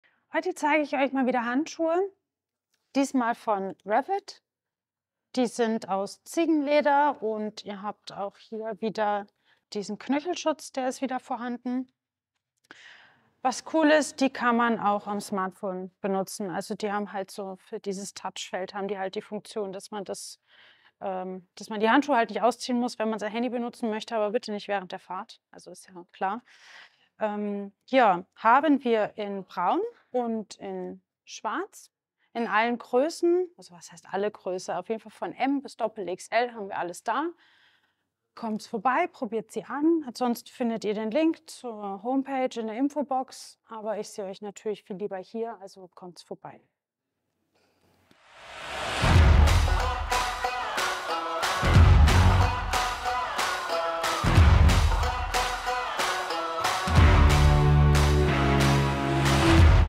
Handschuhe Tracker von REVIT | SIP Scootershop
Hier im Shop erhältlich Klassischer Motorradhandschuh im zeitlosen Design.

Der Tracker Motorradhandschuh von REVIT verbindet klassisches Design mit Rundumschutz. Strategisch eingearbeitete laminierte Reflektoren und leitfähige Fingerspitzen an Daumen und Zeigefinger zur Smartphone-Nutzung verleihen dem Handschuh zusätzlich das gewisse Etwas.

► PWR|Shield auf der Handfläche
► CE-Zertifizierung
► Touchscreen-Funktion
► Um Feuchtigkeit im Handschuh zu vermeiden, wird Tri-Fleece-Futter in der Handschuhinnenseite verwendet.

Das PWR Shield auf der Handfläche sorgt für unübertreffliche Abriebfestigkeit und schützt vor Schnitten und Rissen. Für einen idealen Schutz der Fingerknöchel sorgen die Temperfoam® Knöchelschützer – sicher gegen Aufschläge und dennoch komfortabel.

Eigenschaften im Überblick
► EN 13594 zertifiziert
► Außenmaterial: trommelgefärbtes Ziegenleder
► Tri-Fleece-Futter
► Temperfoam® Fingerknöchel
► Verstelllasche am Bündchen
► Touchscreen-Funktion
► Klettverschluss

Hat dir das Video gefallen? Schau mal in unserem Shop vorbei Hast du Fragen? Dann schreib uns in den Kommentaren.